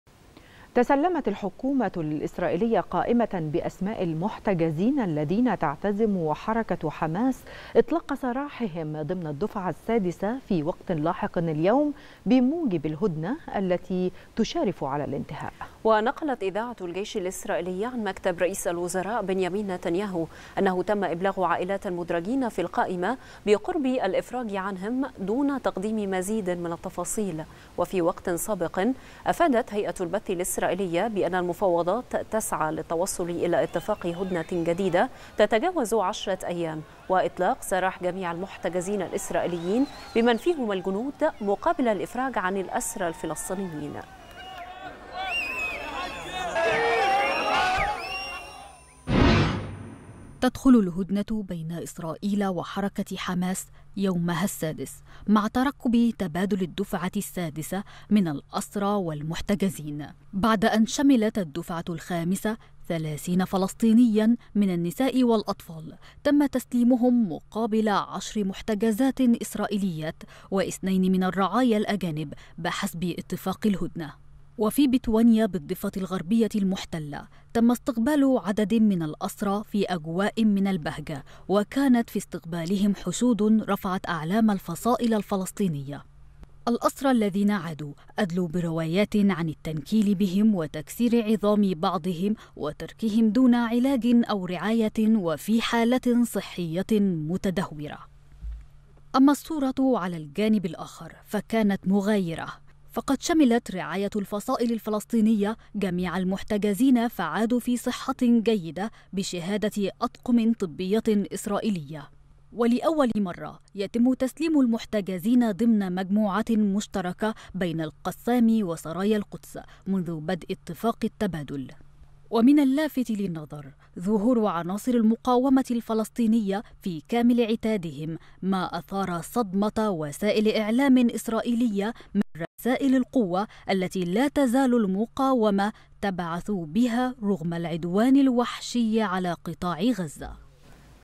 الحكومة الإسـ ـرائيلية تتسلم قائمة بأسماء المحتجزين المزمع إطلاق سراحهم اليوم ضمن الدفعة السادسة
الحكومة الإسـ ـرائيلية تتسلم قائمة بأسماء المحتجزين المزمع إطلاق سراحهم اليوم ضمن الدفعة السادسة بموجب اتفاق الهدنة